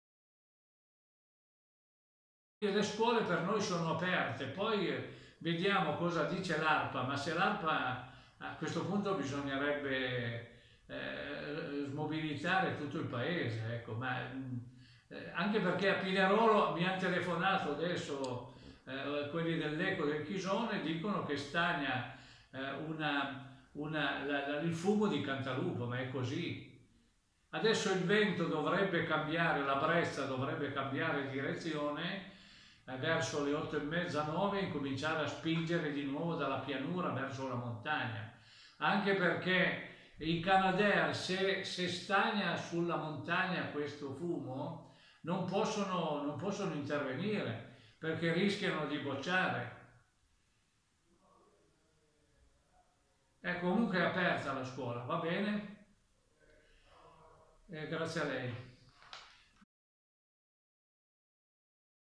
Incendio a Cantalupa: il sindaco Bello risponde ai cittadini
Dopo aver trascorso tutta la notte in municipio per far fronte all'emergenza incendio, il sindaco di Cantalupa Giustino Bello questa mattina risponde alle chiamate dei cittadini che chiedono informazioni sui servizi comunali, come l'apertura delle scuole.